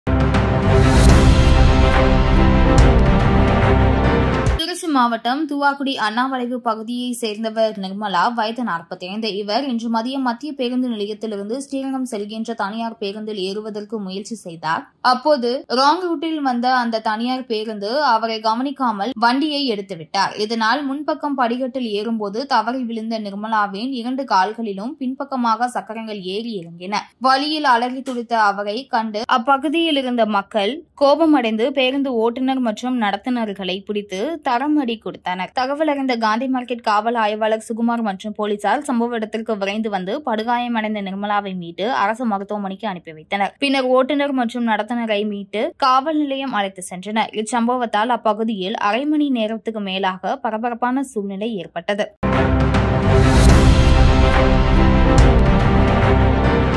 19.5.23 | தனியார் பேருந்து தவறி விழுந்த பெண் கால்களில் ஏறி இறங்கியது - ஓட்டுநருக்கு தர்ம அடி
19.5.23 | தனியார் பேருந்து தவறி விழுந்த பெண் கால்களில் ஏறி இறங்கியது - ஓட்டுநருக்கு தர்ம அடி